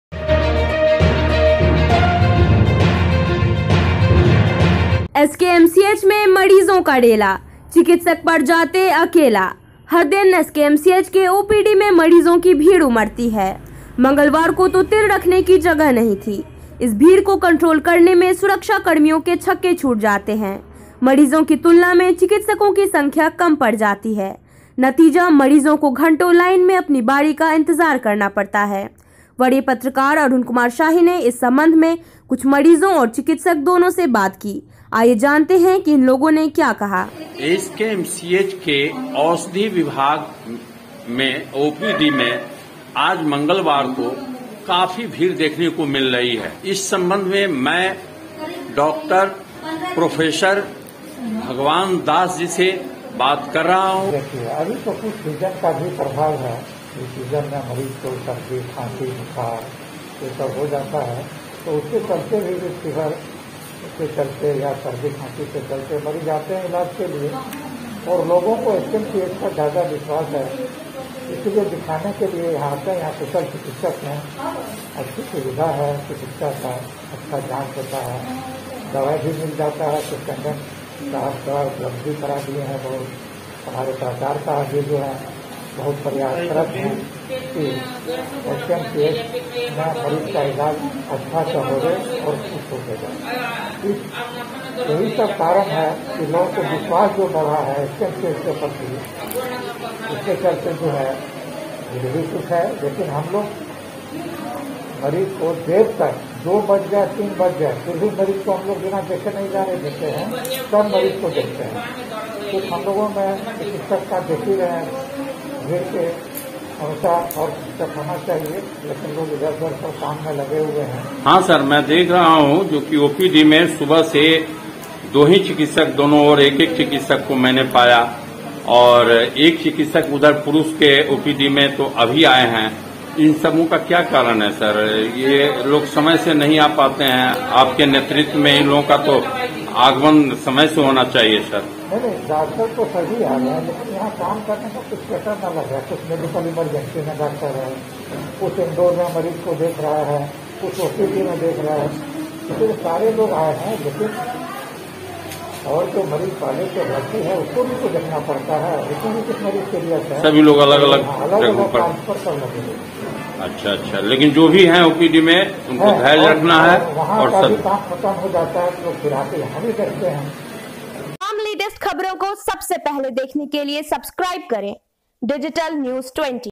SKMCH में मरीजों का रेला, चिकित्सक पड़ जाते 'अकेला'
DN20, Muzaffarpur  : हर दिन एसकेएमसीएच के ओपीडी में मरीजों की भीड़ उमड़ती है। मंगलवार को तो तिल रखने की जगह नहीं थी। इस भीड़ को कंट्रोल करने में सुरक्षाकर्मियों के छक्के छूट जाते हैं। मरीजों की तुलना में चिकित्सकों की संख्या कम पड़ जाती है। नतीजा, मरीजों को घंटों लाइन में अपनी बारी का इंतजार करना पड़ता है। वरीय पत्रकार अरुण कुमार शाही ने इस संबंध में कुछ मरीजों और चिकित्सक दोनों से बात की। आइए, जानें की इनलोगों ने क्या कहा।